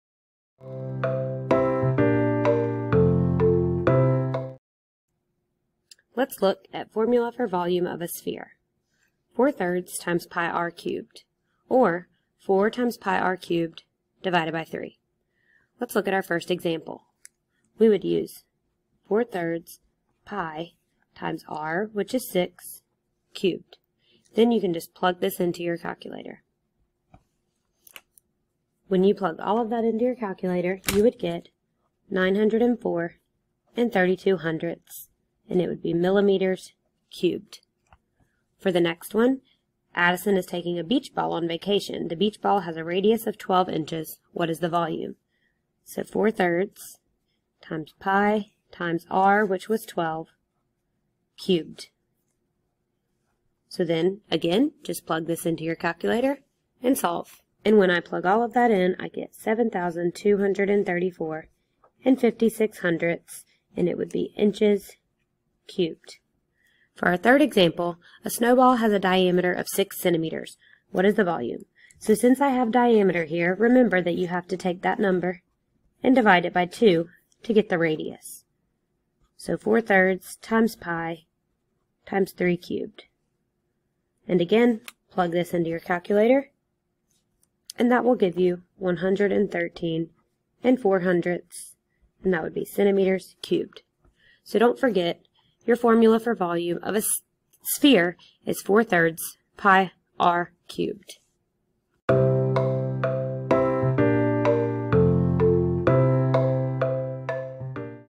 Find the Volume of a Sphere
How to find the volume of a sphere. The formula for volume of a sphere equals,  4/3 pi times radius cubed.  In this video, I work on three example problems and go over the formula for finding the volume of a sphere.

MGSE8.G.9 Apply the formulas for the volume of cones, cylinders, and spheres and use them to
solve real-world and mathematical problems.